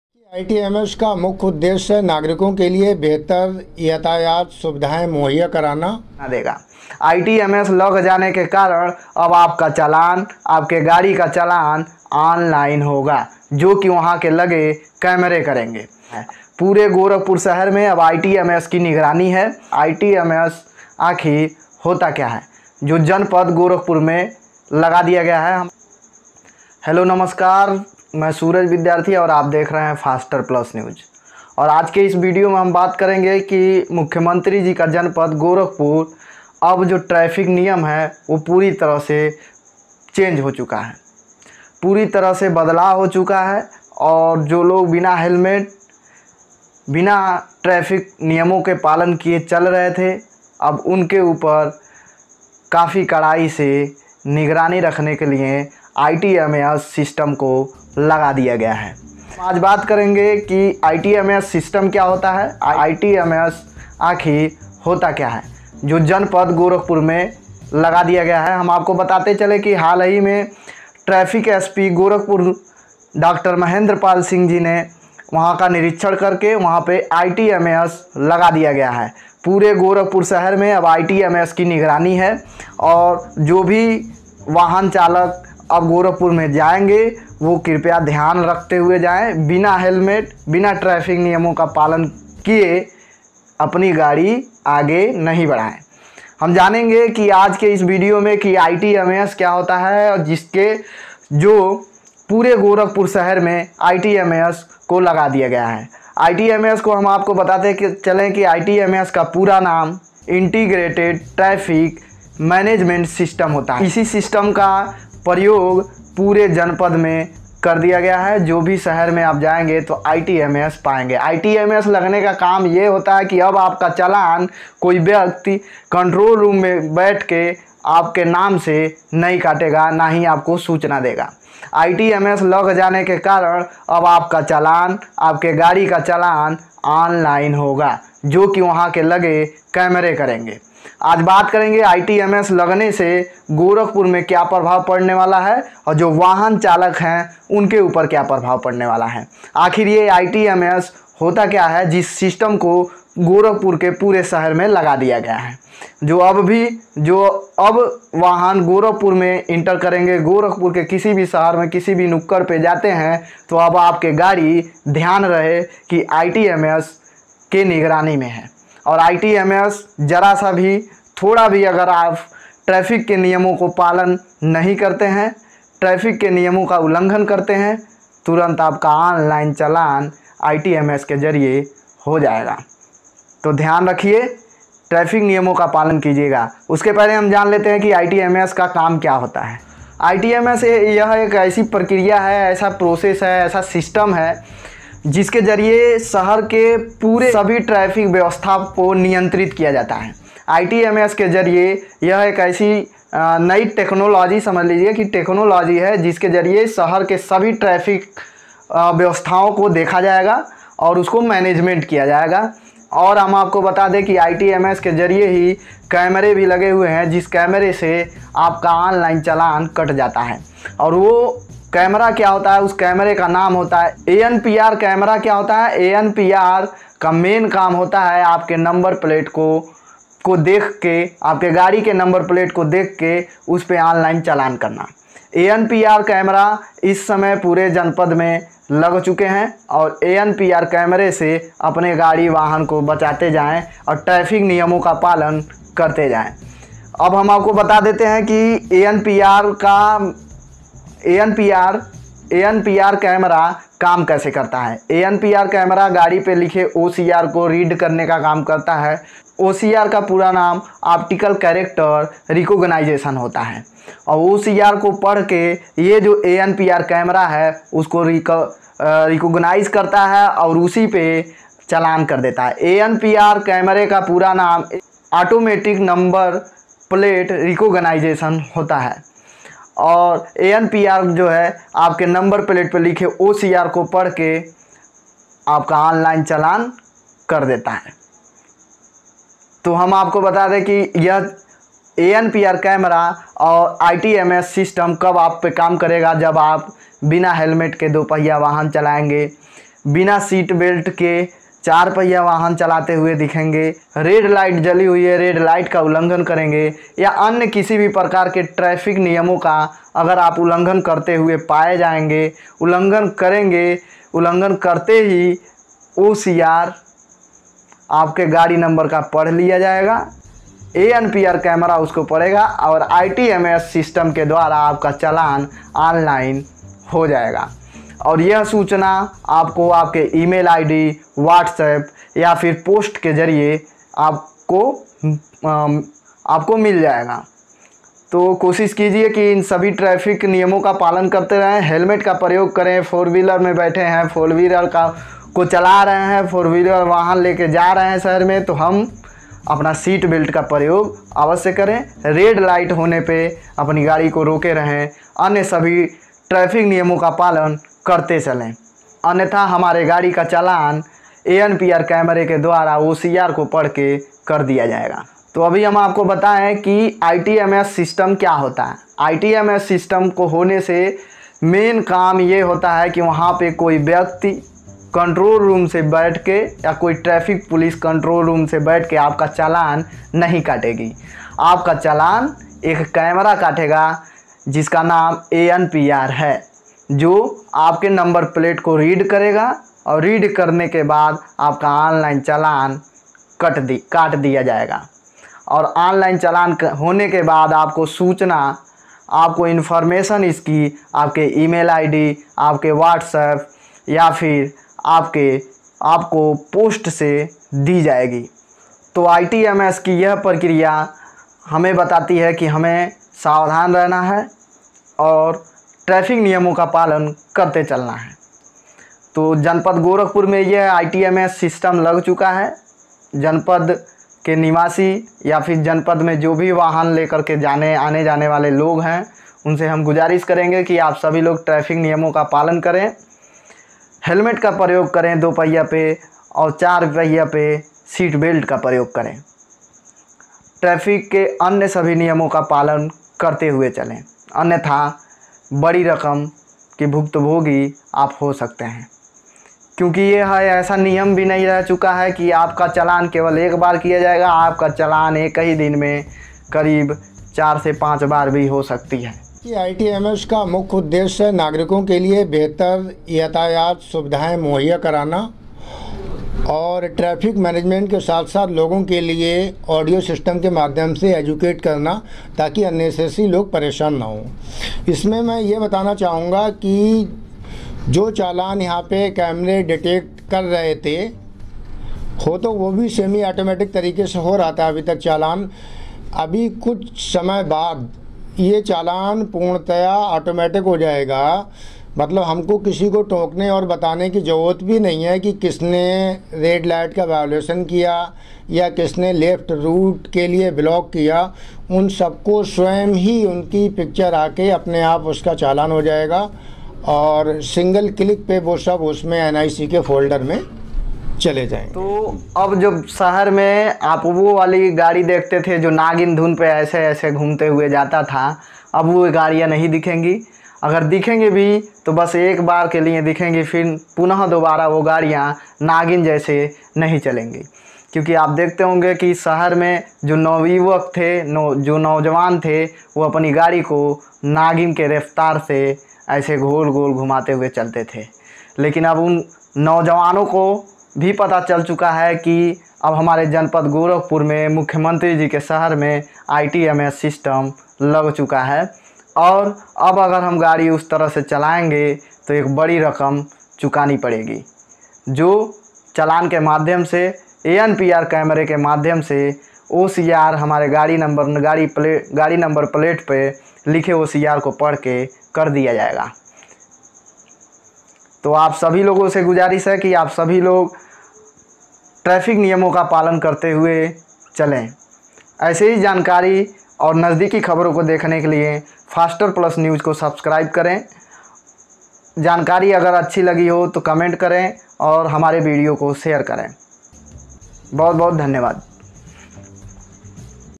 ITMS सिस्टम से कैमरा कैसे करता है ऑनलाइन चालान जाने इस वीडियो से पूरी जानकारी
ITMS सिस्टम से कैमरा कैसे करता है ऑनलाइन चालान जाने इस वीडियो से पूरी जानकारी ग्रुप में ऑनलाइन चालान एनपीआर कैमरे से शुरू कर दिया गया है जिसकी सूचना ईमेल आईडी व्हाट्सएप और पोस्ट से दिया जायेगा